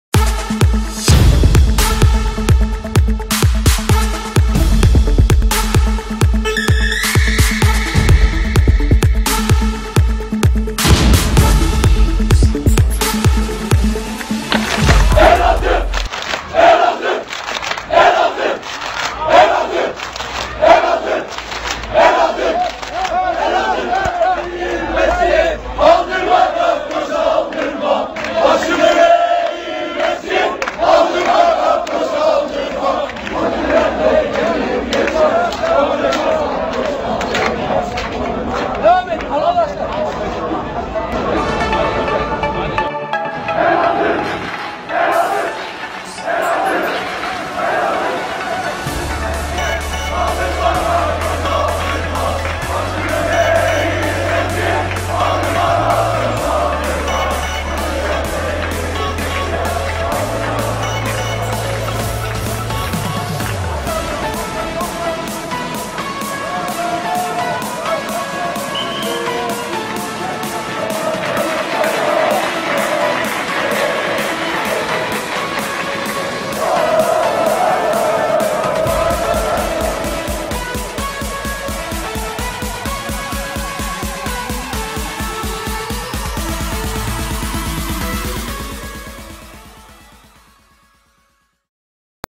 Elazığspor-Sivasspor kardeşliği-Yigidolar'dan Elazığspor'a moral
Elazığspor-Sivasspor kardeşliği-Yigidolar'dan Gakgolar'a moral ziyareti